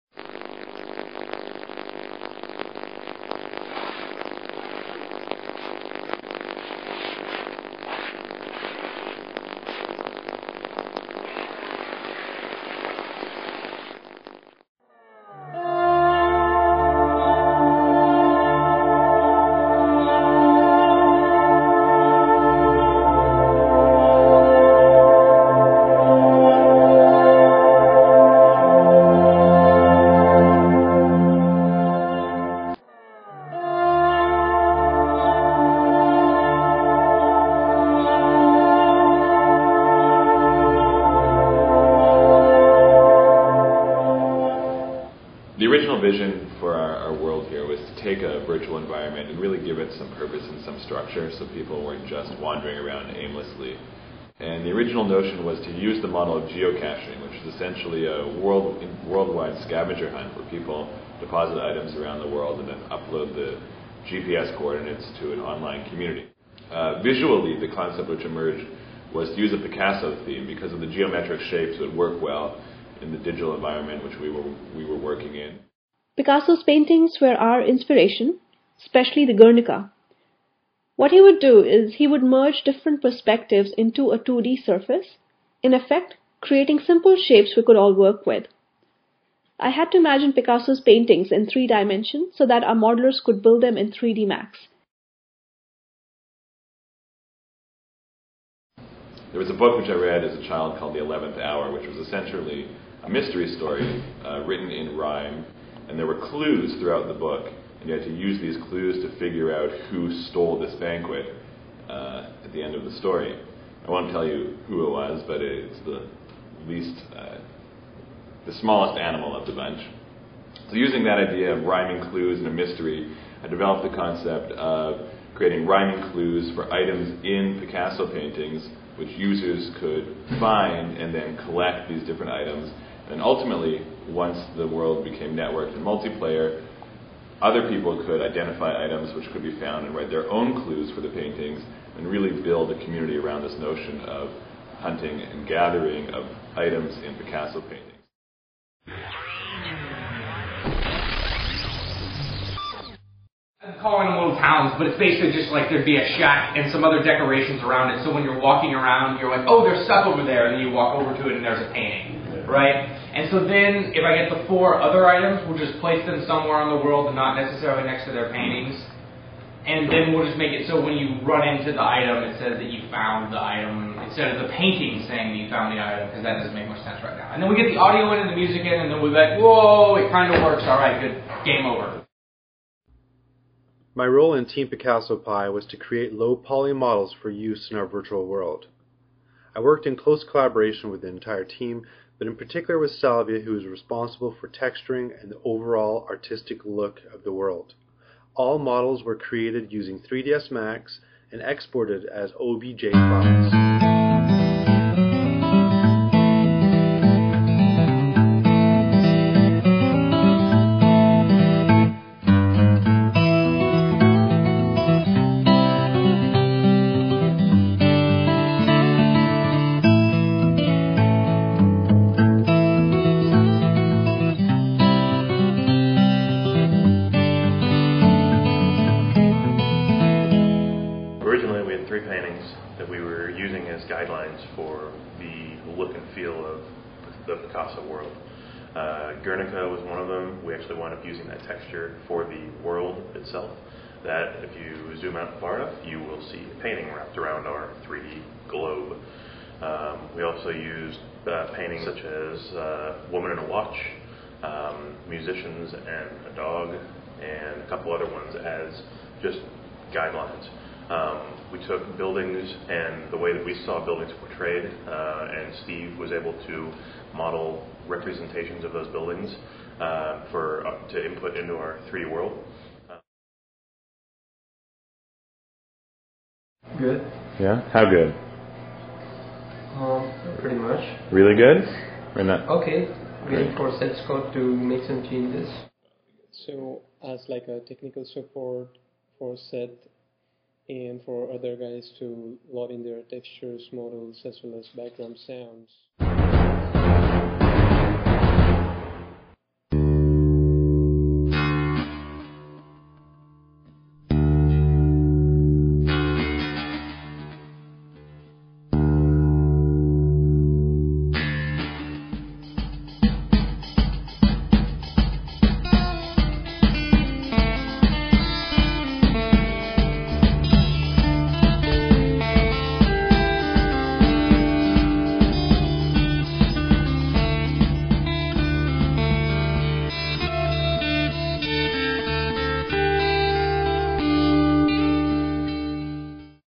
Projects 1-Abstraction-Centre for Digital Media (CDM)
Abstraction:The Making of Picasso's Pi Virtual World Project.

Picasso's Pi, is a virtual world student project built in Java.